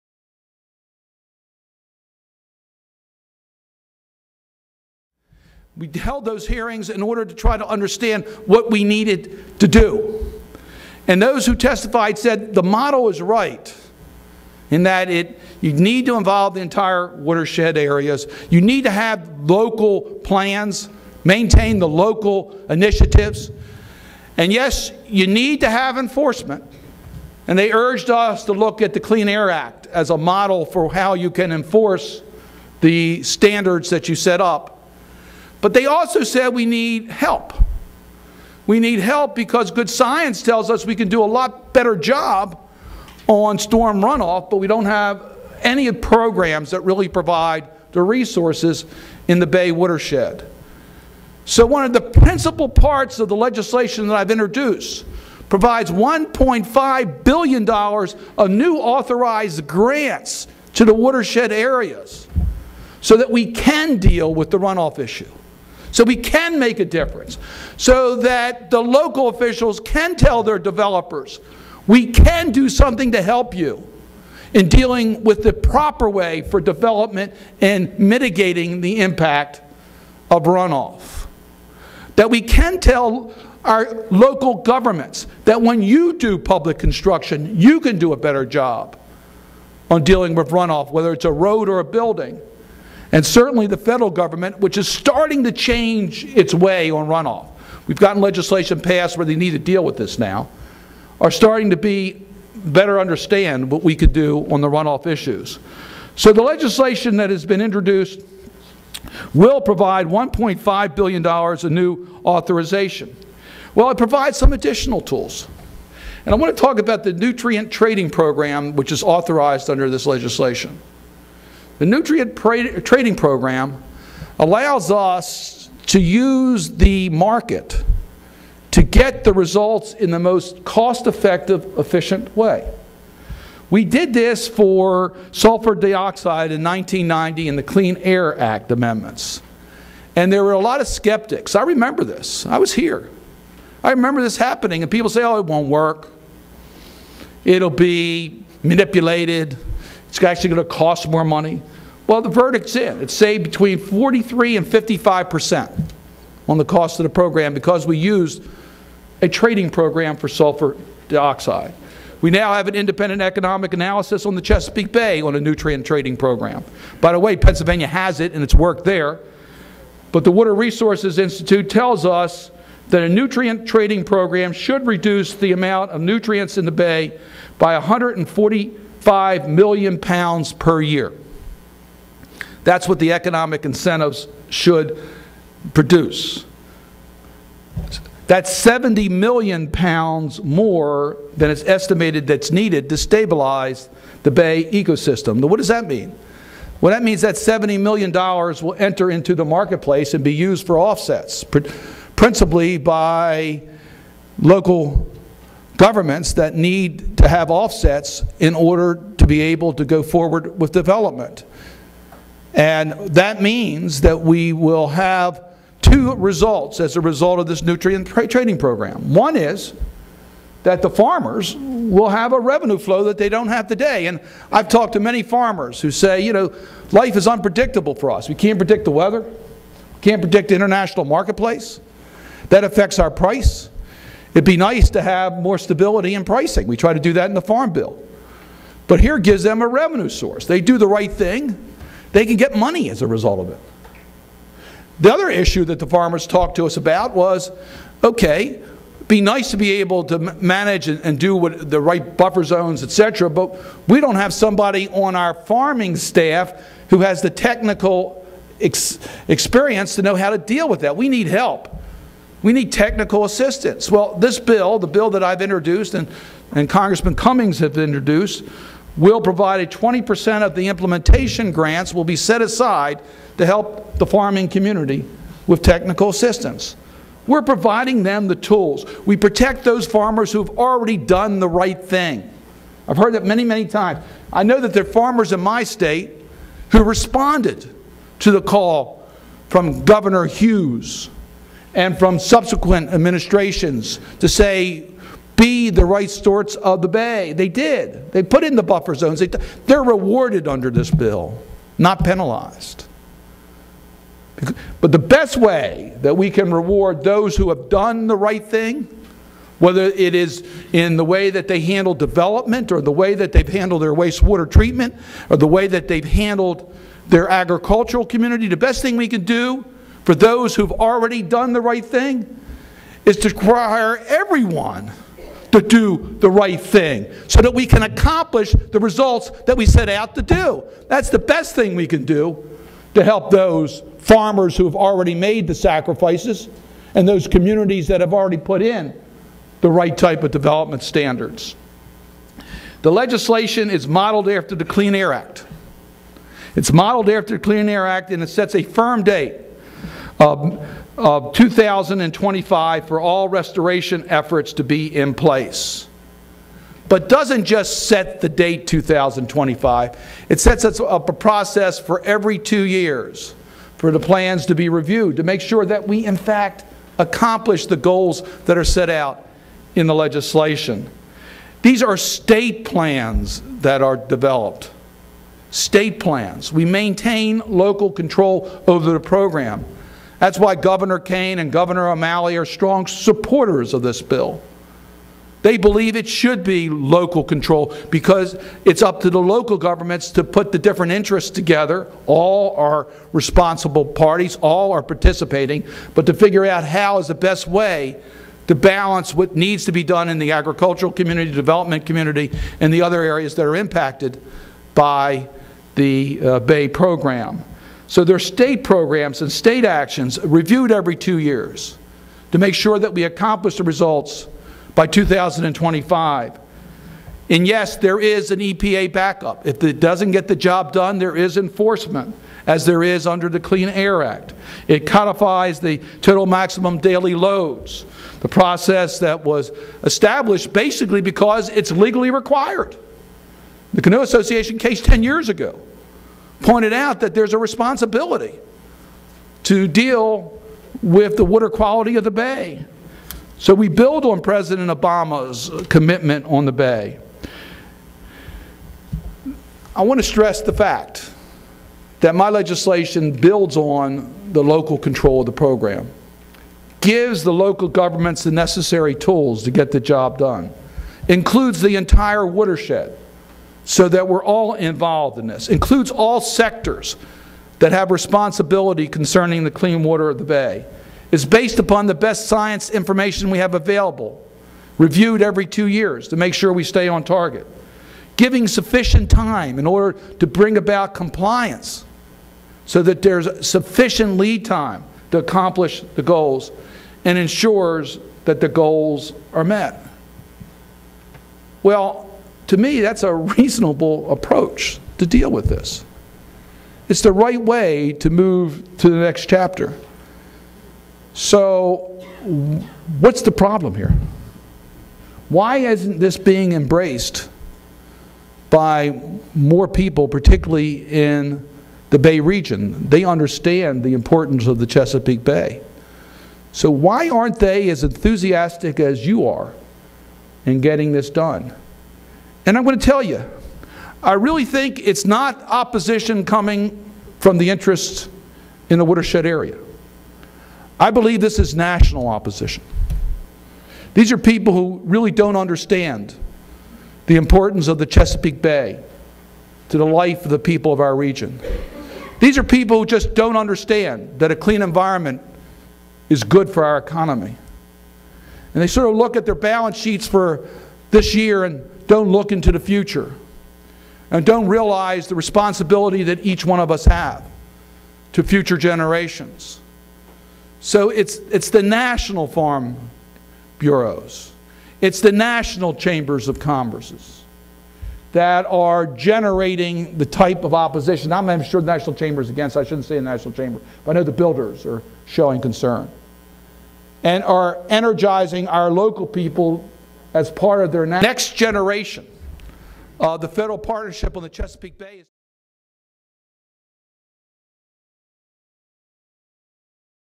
Senator Ben Cardin CleanH2o Pt3.mov
Part 3 of Senator Ben Cardin presentation addressing the Clean Water Coalition at the 1st Annual conference